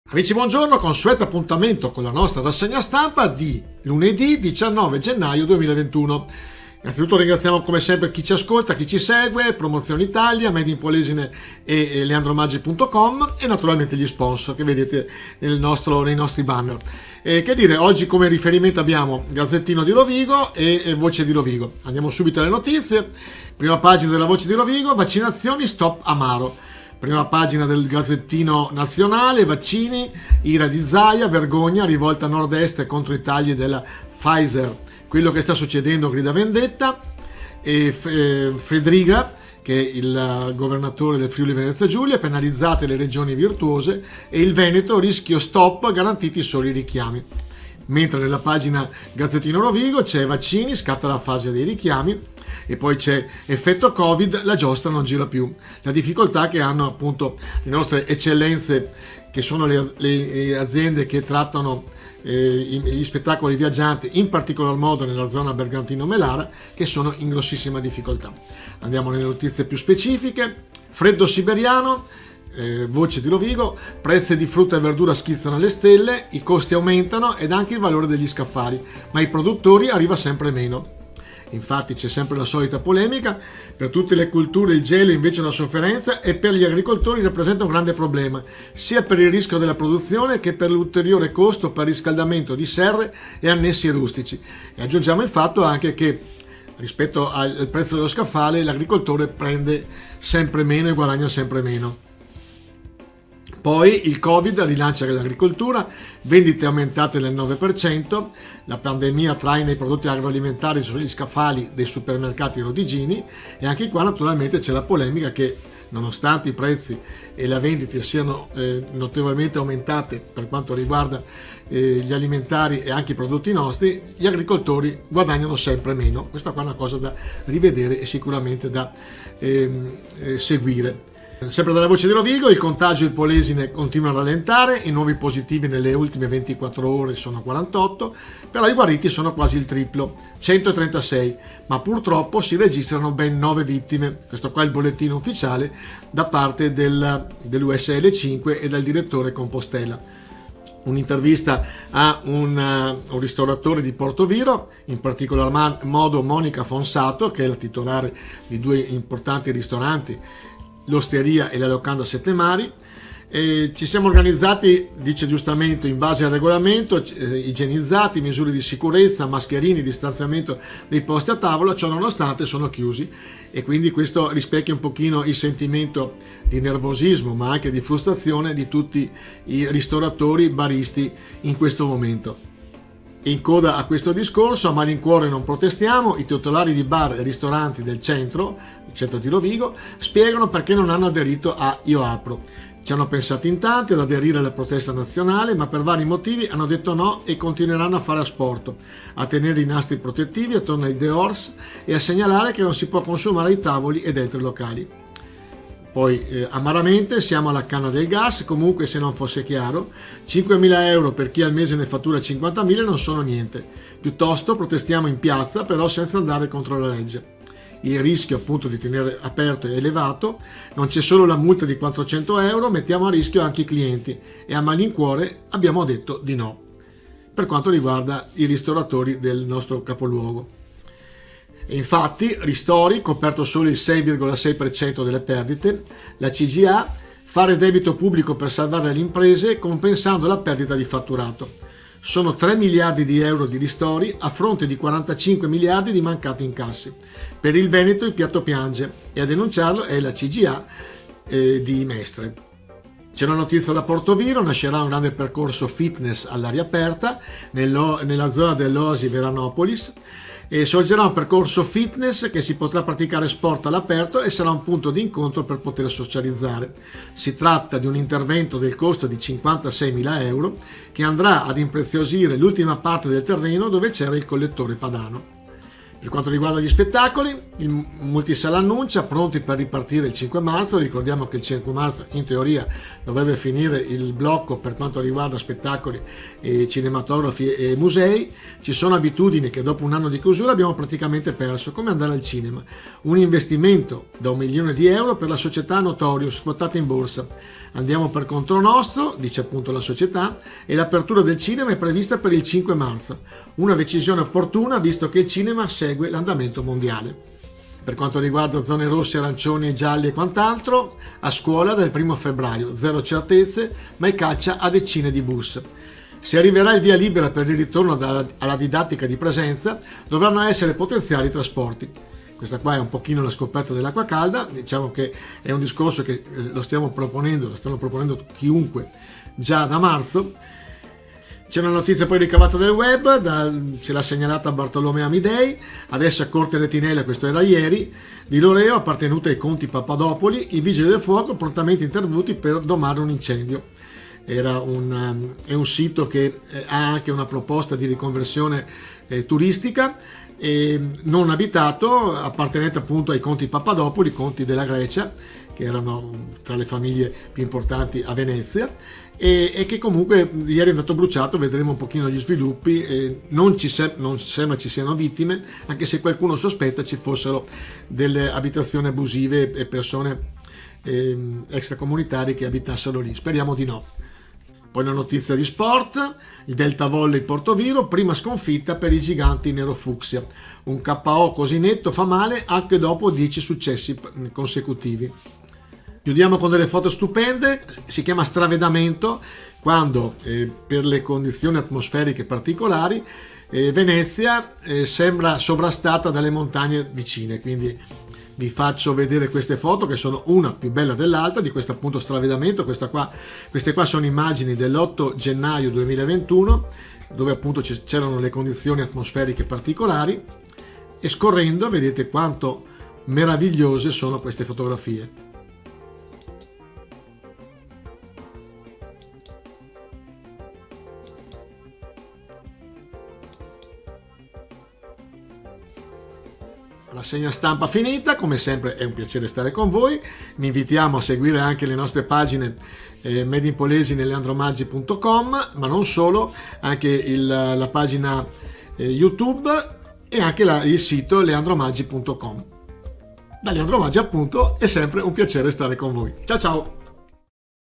Promozione Italia Rassegna stampa del 18 gennaio 2021
Rassegna stampa del 18 gennaio 2021  offerta dall'Associazione Promozione Italia e Made in Polesine.
In studio Leandro Maggi
Sponsor della puntata di oggi : Ottica Moderna Taglio di Po ( Ro )